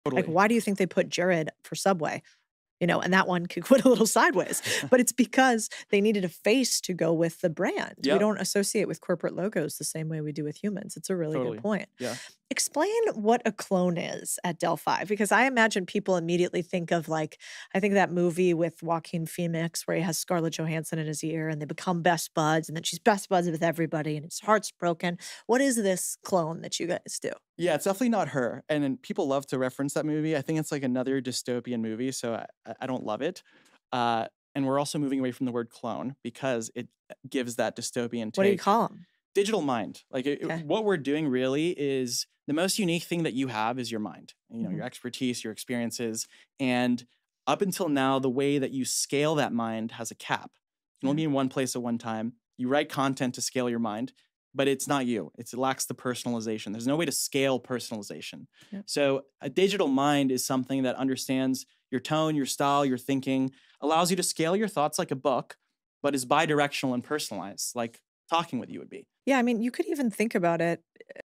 Scale Your Thoughts: The Digital Mind Explained!
How can tech scale human expertise and personalize digital interactions? Discover how 'digital minds' could revolutionize content, offering personalized experiences. Is this the future, or a step too far? FULL VIDEO